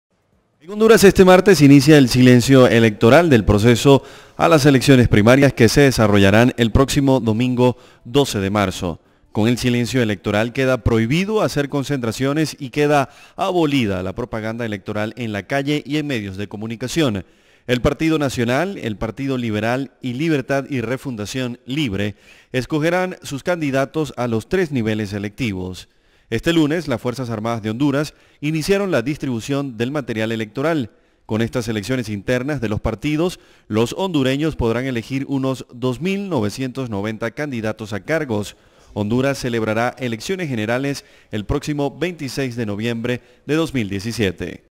Comenzó silencio electoral de cara a elecciones primarias en Honduras
Todo está listo para las primarias en Honduras,  proceso para cual  los partidos políticos llevan a cabo sus elecciones internas. De hecho este martes inició el silencio electoral del proceso de las primarias, que se desarrollarán el próximo domingo 12 de marzo.

Con el silencio electoral queda prohibido hacer concentraciones y queda abolida la propaganda electoral en la calle y en medios de comunicación.

Gustavo Velazco / Redacción CB24